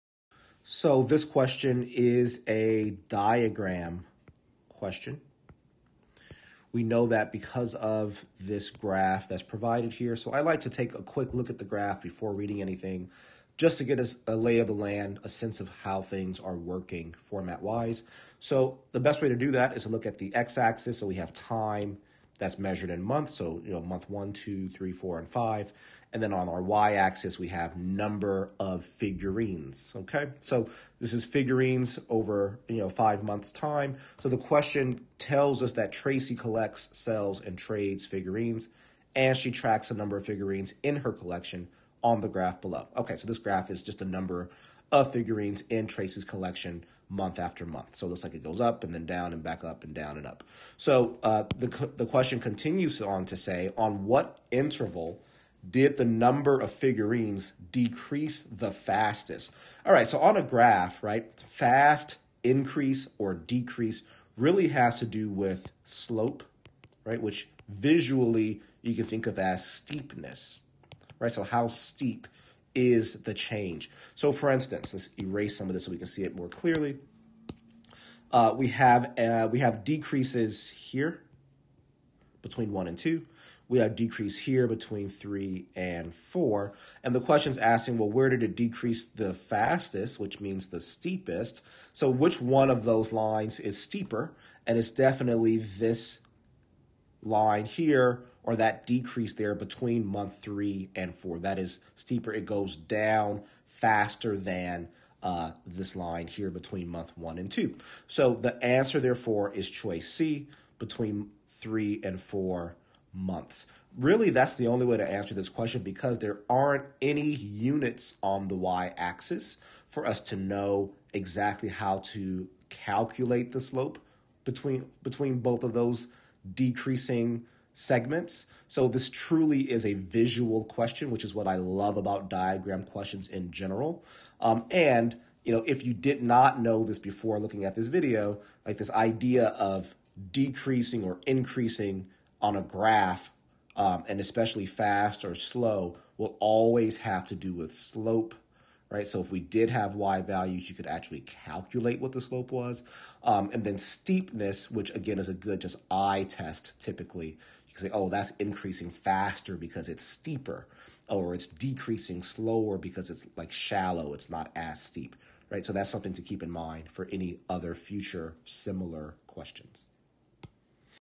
Tracy collects, sells, and trades figurines, and she tracks the number of figurines in her.....
Official SAT Practice Test 8, Section 4, Question 2:

Tracy collects, sells, and trades figurines, and she tracks the number of figurines in her collection on the graph below.  On what interval did the number of figurines decrease the fastest?